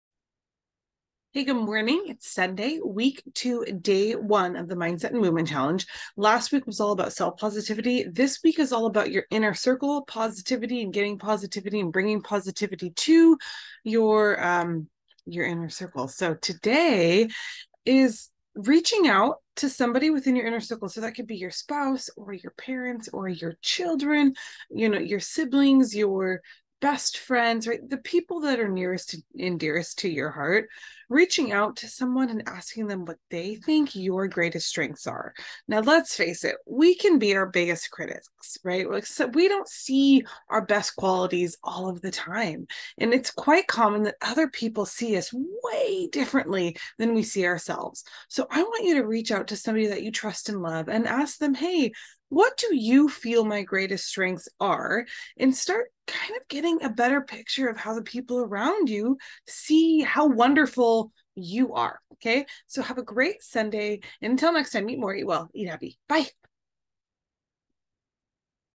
Mindset and Movement Challenge, Week 2 Day 1: Ask about your strengths
This week is about bringing positivity and growth from your inner circle, the people nearest and dearest to your hearts: spouses, children, parents, best friends, siblings, etc.

Let's be honest: we rarely see ourselves the way others do. We can be our worst critics.

Today, reach out to someone whom you trust and love and ask them what they feel are your strengths. How do others see the amazing you?